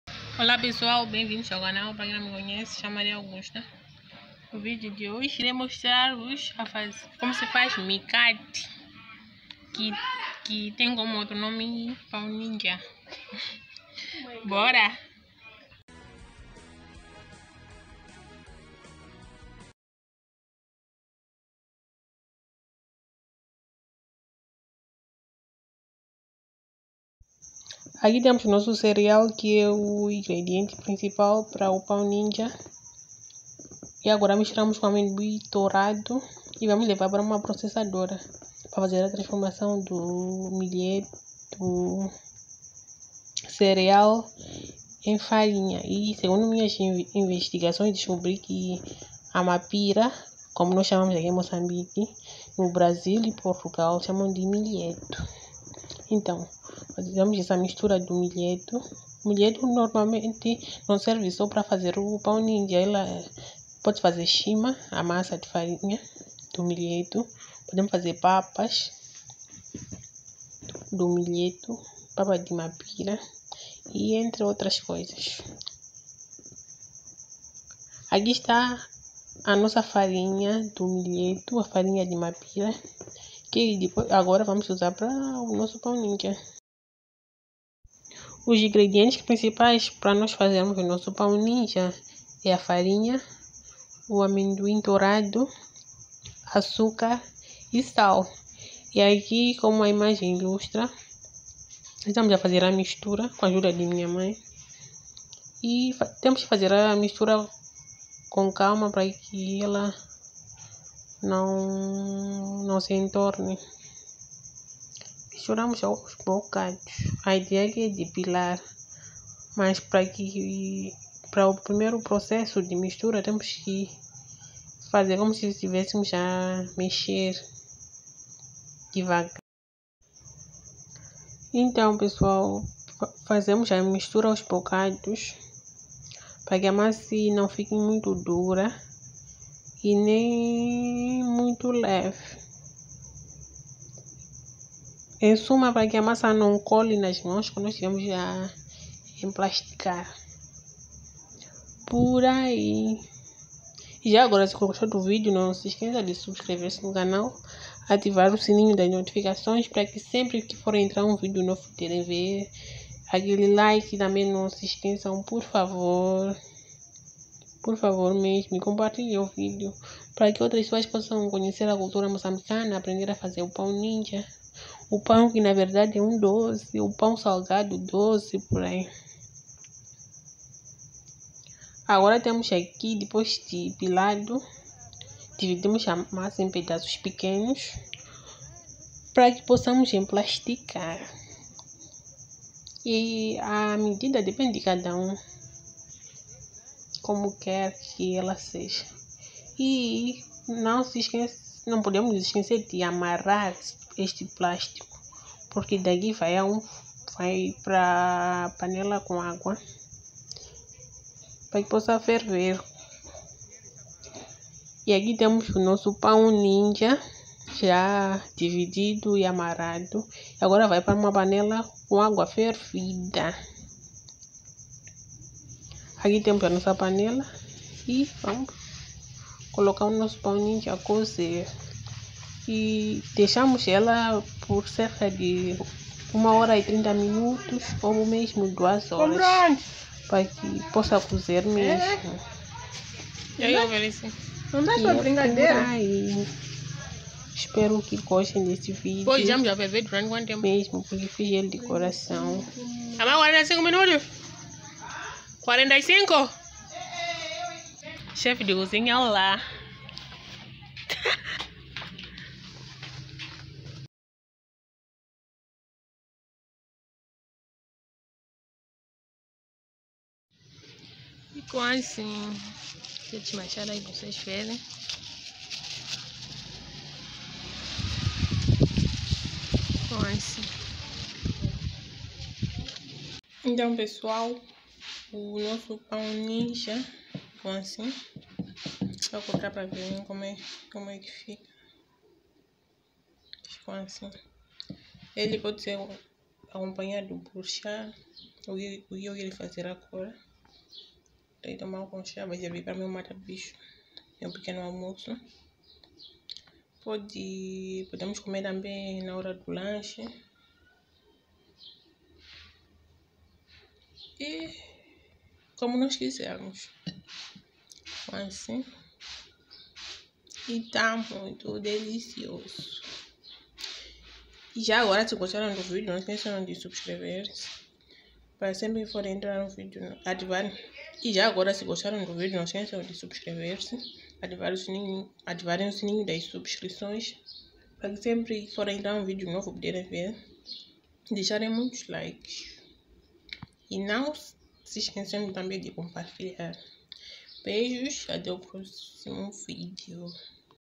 PRATO TÍPICO DE MOÇAMBIQUE 🇲🇿 || PÃO NINJA = SABORES DA TERRA
Olá PESSOAS ❤️!

Espero que esteja tudo, neste vídeo venho compartilhar com vocês como fazemos o nosso PÃO NINJA, espero que gostem.

Não esqueçam de:

SUBSCREVER-SE;
ACTIVAR O SINO 🔔;
E DE COMPARTILHAR.

REDE SOCIAL

DEUS É BOM Á TODO MOMENTO.